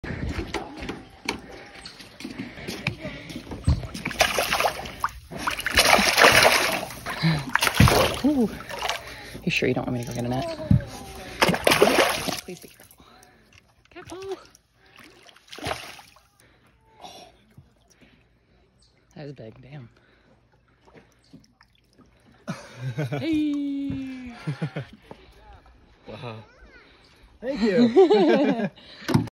Caught A BIG BASS From The Dock 🎣
This big ole bass hit the YUM dinger when as soon as it hit bottom

I’ve targeted bass a handful of times this year which is unusual for me. A few more bass outings before switching gears to staging salmon!

We are a couple weeks out from Lake Superior Provincial Park trip! Any tips on fishing that area would be much appreciated 🎣.

 @Time on the Lake